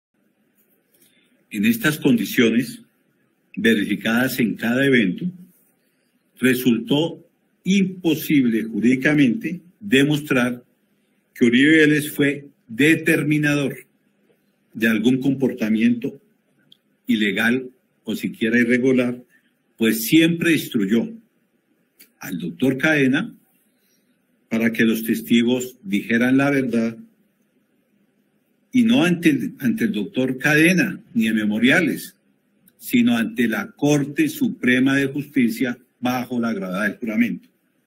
ES IMPOSIBLE DEMOSTRAR QUE ÁLVARO URIBE SEA DETERMINADOR DE ALGÚN COMPORTAMIENTO ILEGAL O IRREGULAR
UstedQuéOpina LA FISCALIA CONCLUYE QUE ES IMPOSIBLE DEMOSTRAR QUE EL EXPRESIDENTE ÁLVARO URIBE SEA DETERMINADOR DE ALGÚN COMPORTAMIENTO ILEGAL O IRREGULAR

Afirma el ente acusador que el expresidente: "siempre instruyó al doctor Cadena para que los testigos dijeran la verdad y no ante el doctor Cadena ni en memoriales, sino ante la Corte Suprema de Justicia bajo la gravedad de juramento"